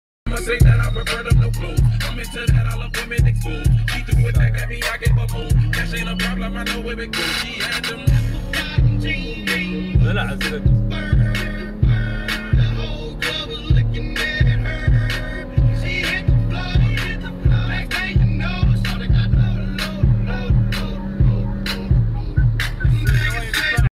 350z vs charger r/t in kuwait
im in pathnfinder my speed 120km